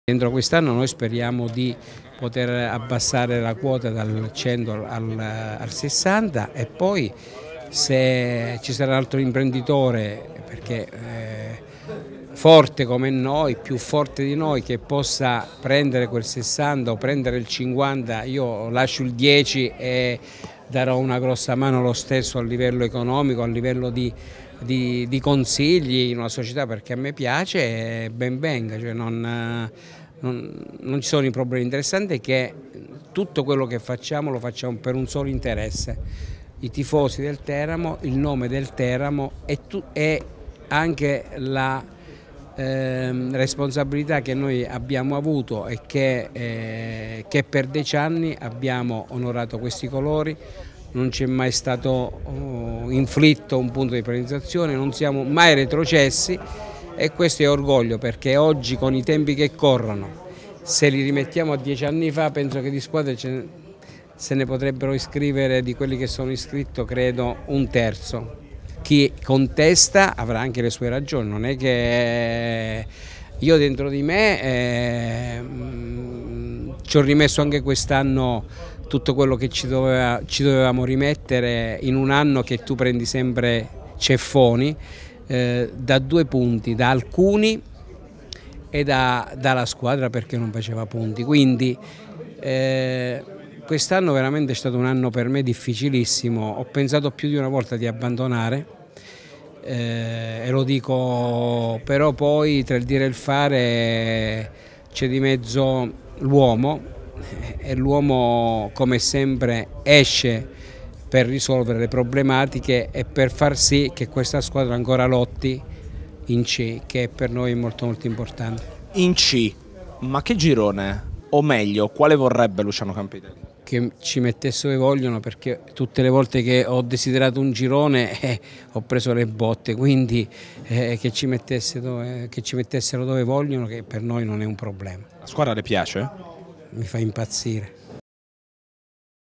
Campitelli 'Il girone Ci mettessero dove vogliono'
A margine della presentazione dei nuovi soci del Teramo, il patron biancorosso Luciano Campitelli ne ha approfittato anche per fare un bilancio delle stagioni passate. Giudizio positivo della squadra che è stata costruita mentre al numero 1 del Diavolo non interessa in quale raggruppamento sarà inserita la sua compagine